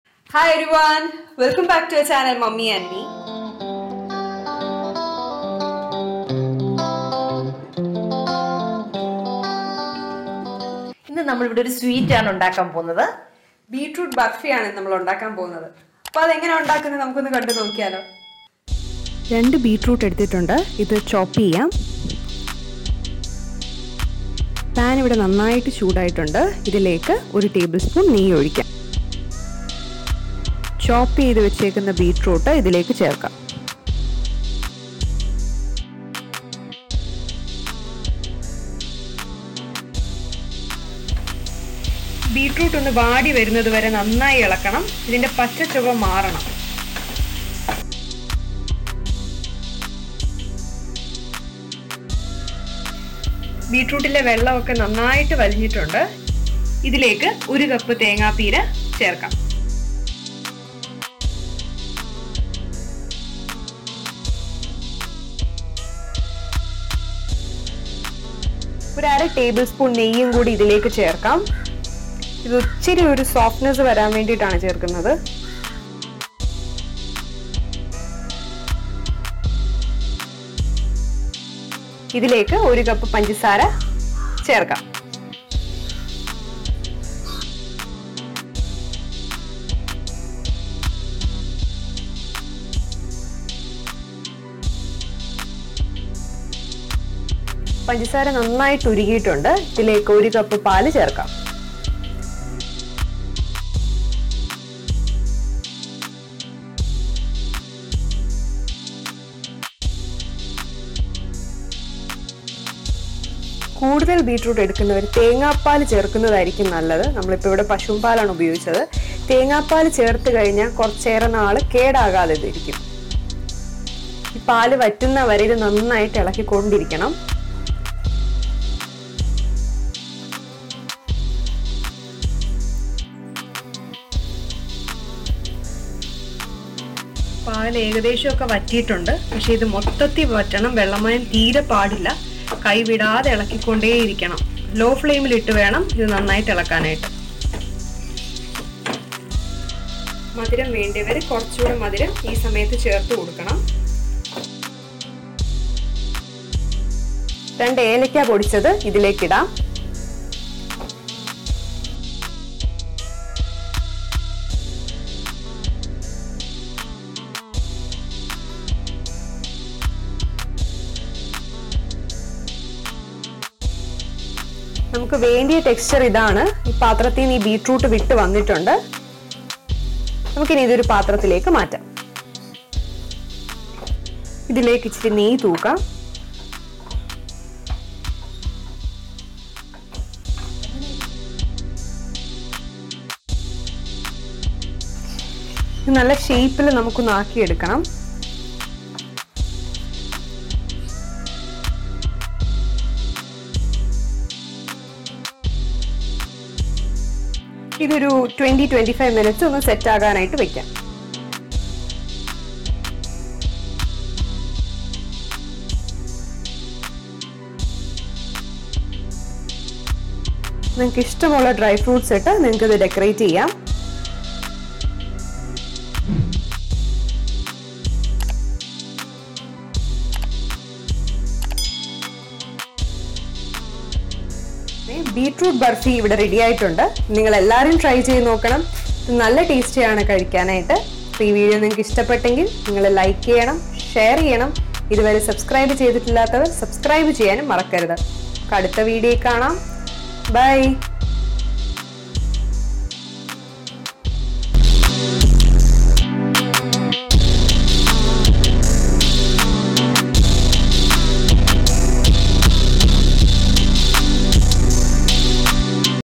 Beetroot Burfi | Few Ingredients | Easy Recipe | Tasty & Healthy | അടിപൊളി ബീറ്റ്റൂട്ട് ബർഫി
Ingredients
Beetroot- 2 nos
Ghee - 2 tbs
Grinded coconut - 1 nos